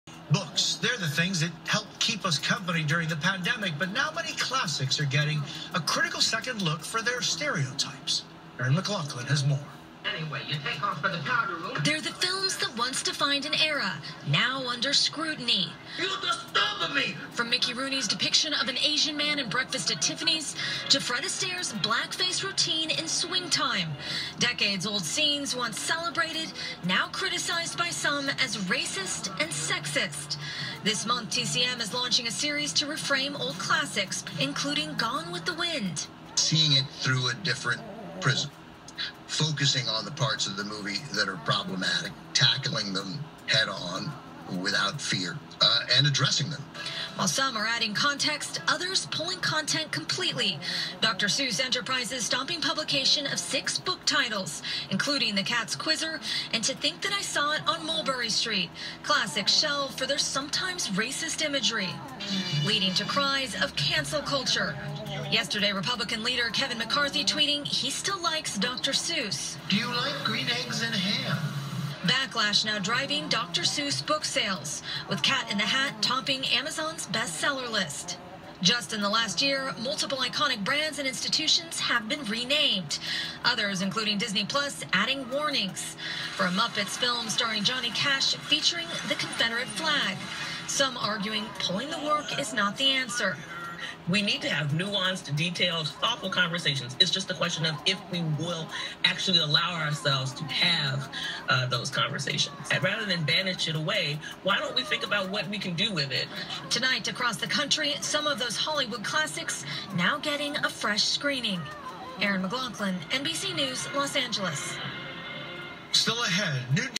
Special Report:Cancel Culture 2021
NBC report on today’s deletions and what’s to come